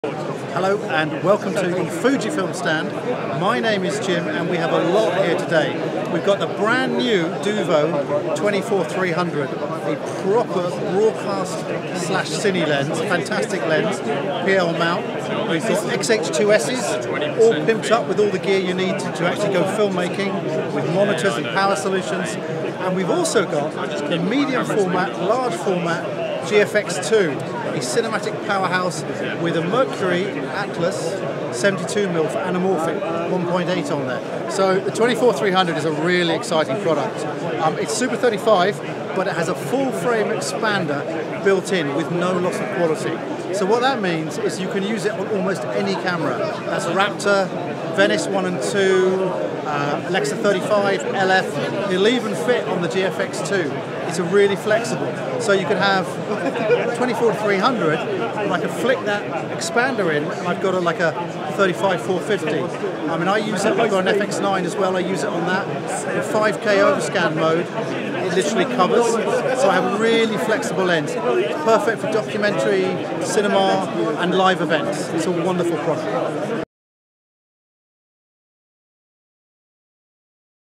[BSC Expo 2024] Duvo - FujiFilm 24-300mm T2.9
The Duvo 24-300mm is a portable TV lens with unparalleled zoom, speed and size in combination. A huge innovation in the format